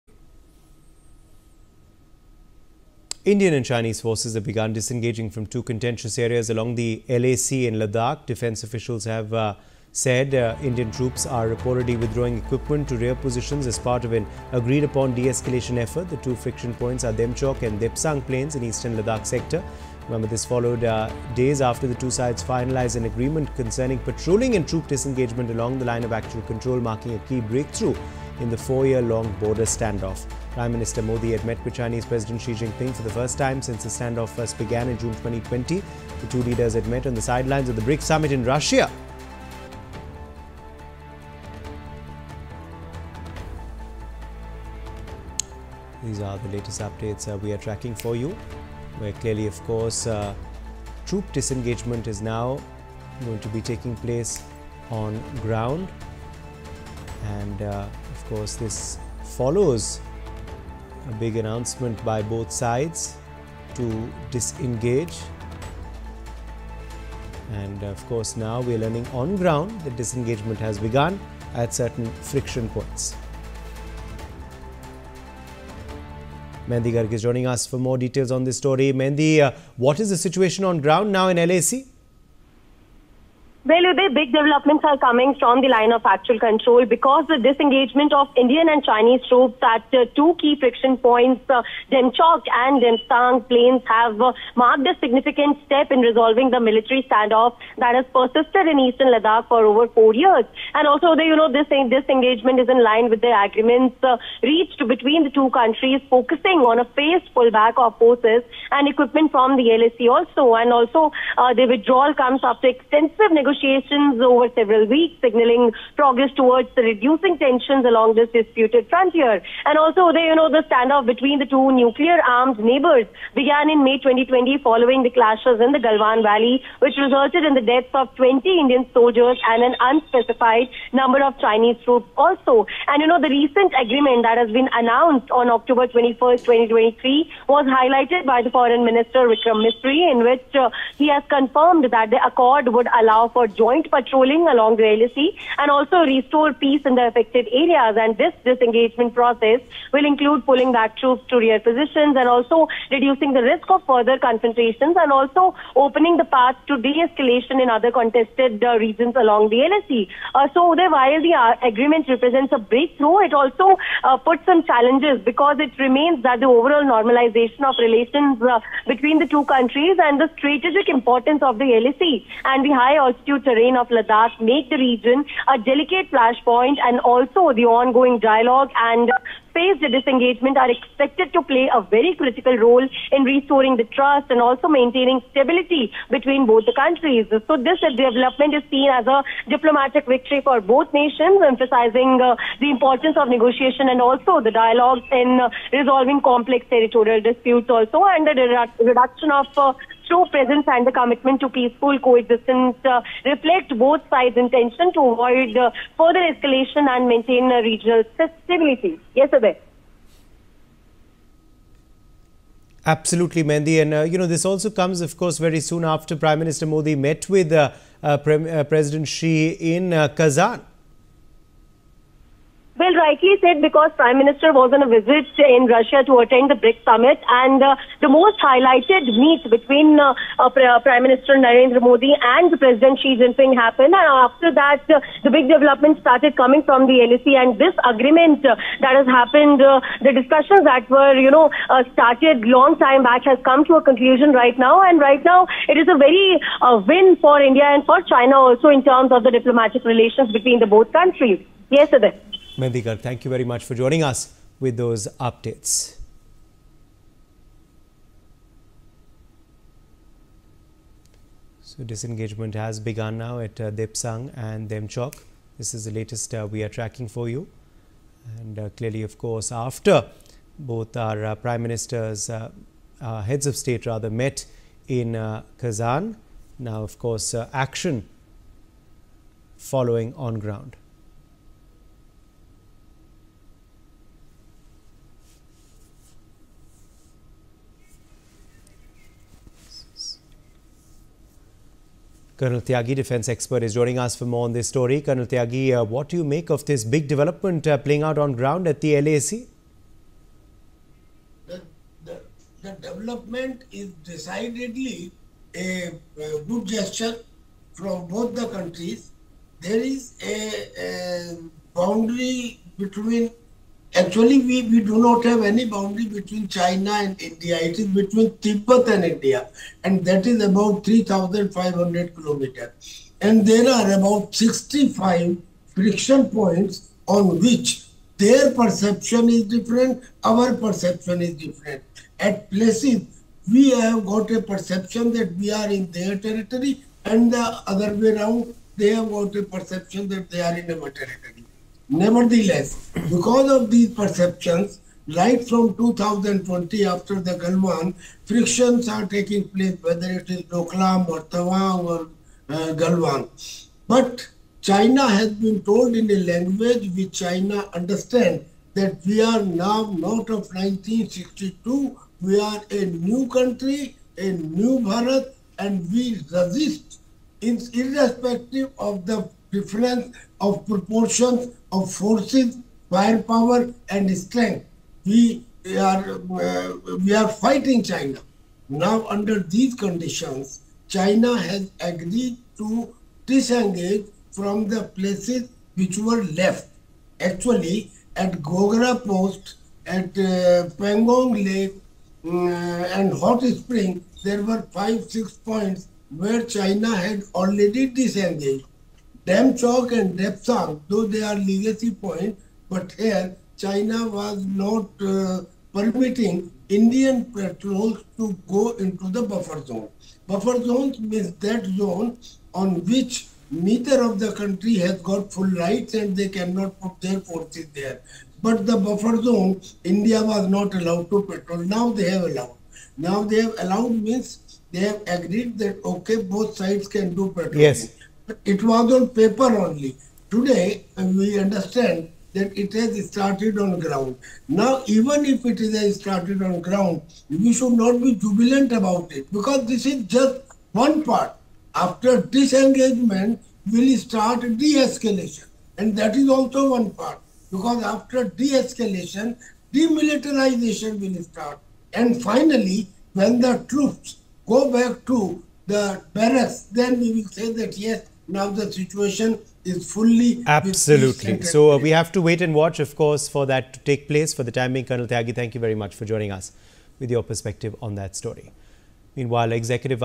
Has Indian-Chinese Troop Disengagement Started in Eastern Ladakh’s Key Friction Points? | NewsX
The disengagement of Indian and Chinese troops has commenced at two significant friction points—Demchok and Depsang Plains—in eastern Ladakh. Defense officials confirm that, following mutual agreements, Indian forces have begun repositioning their equipment to rear areas, signaling a step toward easing tensions in the region. This development marks a pivotal phase in the diplomatic efforts to stabilize the border situation, as both countries work to reduce long-standing tensions and secure peaceful coexistence along the Line of Actual Control.